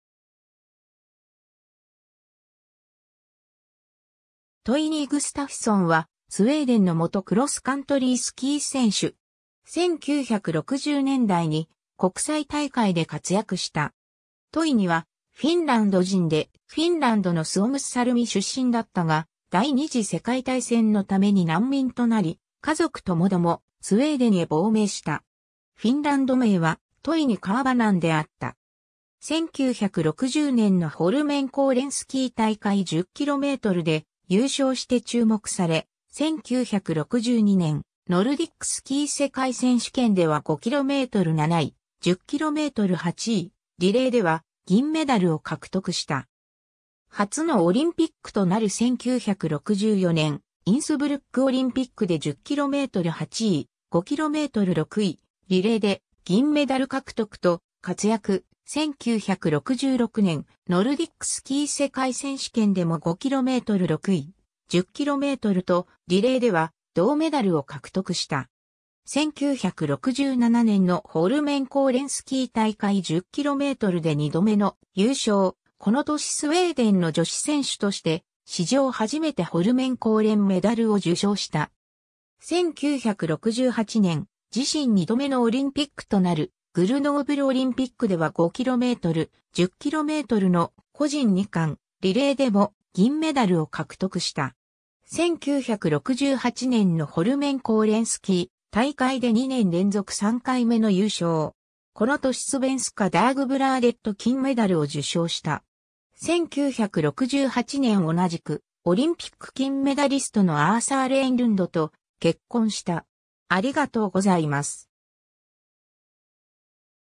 トイニ・グスタフソン
トイニ・グスタフソン（Toini Gustafsson-Rönnlund、1938年1月17日 - ）はスウェーデンの元クロスカントリースキー選手。
1960年代に国際大会で活躍した。
 トイニはフィンランド人でフィンランドのスオムッサルミ出身だったが、第二次世界大戦のために難民（いわゆるフィンランド戦争の子供たちFinnish war children）となり、家族ともどもスウェーデンへ亡命した。
フィンランド名はToini Karvonenであった。
 1960年のホルメンコーレンスキー大会10kmで優勝して注目され、1962年ノルディックスキー世界選手権では5km7位、10km8位、リレーでは銀メダルを獲得した。
 初のオリンピックとなる1964年インスブルックオリンピックで10km8位、5km6位、リレーで銀メダル獲得と活躍、 1966年ノルディックスキー世界選手権でも5km6位、10kmとリレーでは銅メダルを獲得した。
 1967年のホルメンコーレンスキー大会10kmで2度目の優勝、この年スウェーデンの女子選手として史上初めてホルメンコーレン・メダルを受章した。
 1968年、自身2度目のオリンピックとなるグルノーブルオリンピックでは5km、10kmの個人2冠、リレーでも銀メダルを獲得した。
1968年のホルメンコーレンスキー大会で2年連続3回目の優勝。
この年スヴェンスカ ダーグブラーデット金メダル（Svenska Dagbladet Gold Medal、スウェーデンの最優秀スポーツ選手賞）を受賞した。
 1968年同じくオリンピック金メダリストのアーサー・レーンルンドと結婚した。